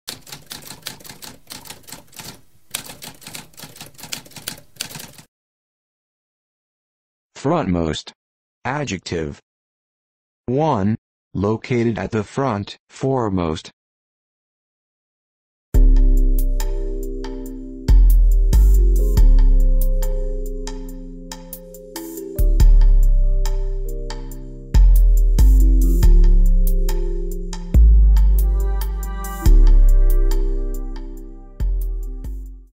What does frontmost mean?
A spoken definition of frontmost.

Intro Sound:
Typewriter - Tamskp